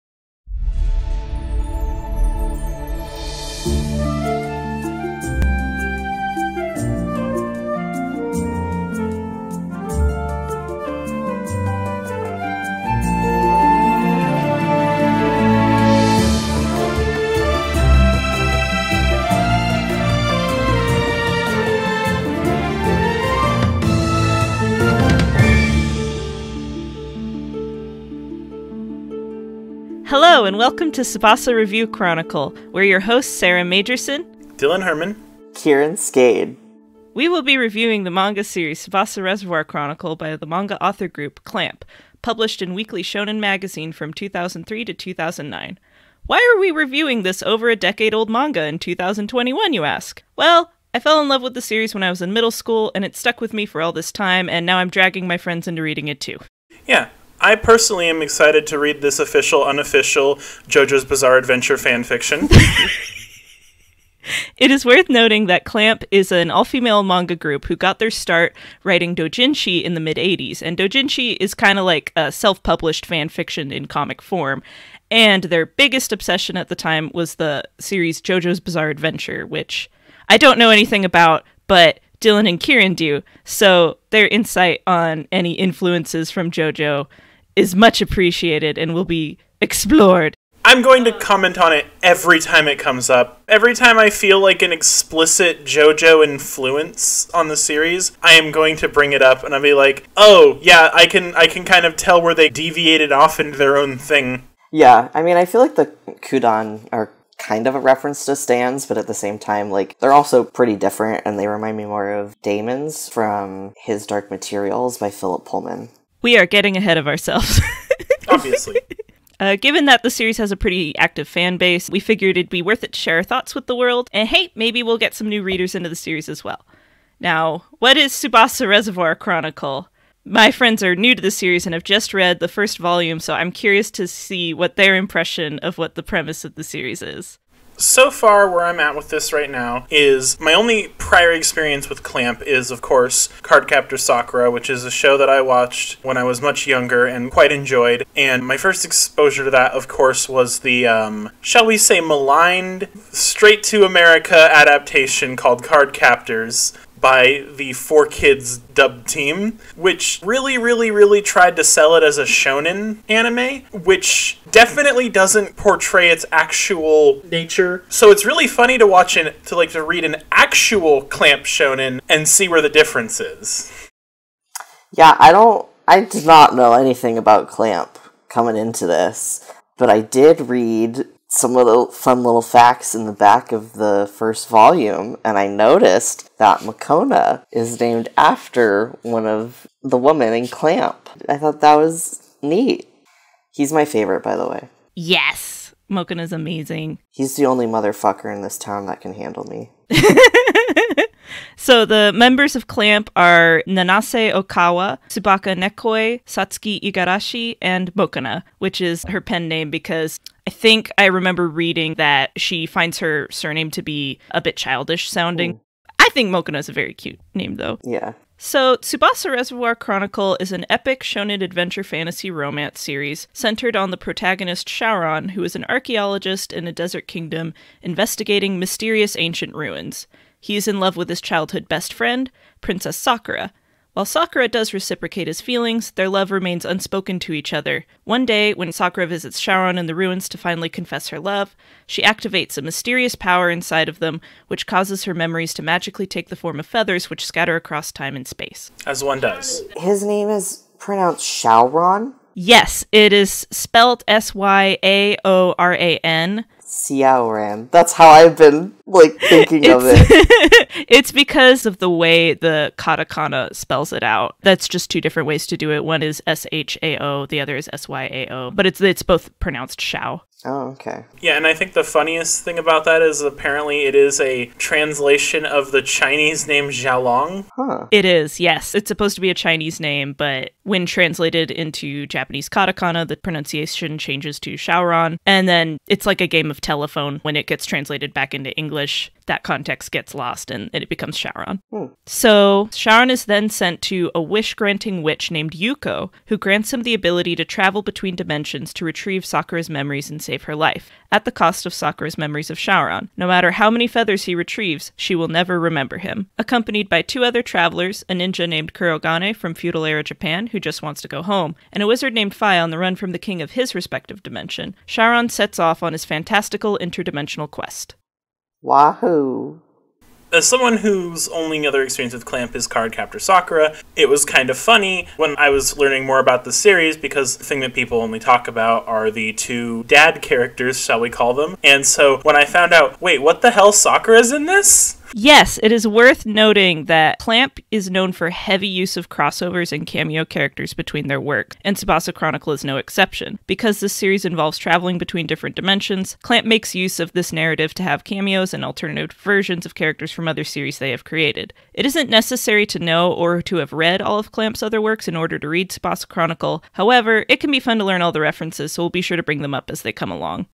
Tsubasa Review Chronicle: Volume 1 (Preview - First Six Minutes)
Warning for mild language.

This is a preview of the first six minutes of the pilot episode of Tsubasa Review Chronicle, a podcast in which Sarah Majercin (CLAMP expert), Dillon Herman, and Kieran Skade (new readers, and Jojo's Bizarre Adventure experts) read and review the shounen manga series Tsubasa Reservoir Chronicle by CLAMP. The first episode will cover the introduction of the podcast, as well as a review of the first volume.